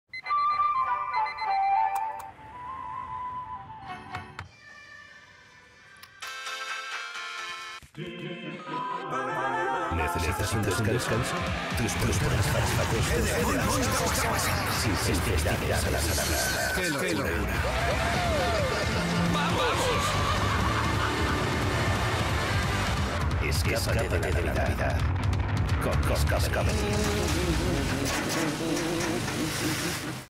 Escapa de la navidad con Discovery Channel
¿Cansado de la navidad?¿Necesitas un descanso? Tus programas favoritos sin festividades a la vista, escápate de la navidad con Discovery Channel

👀Los mejores programas en nuestro canal 👉 @DiscoveryEspana
🛎 ¡Activa las notificaciones y no te pierdas nada!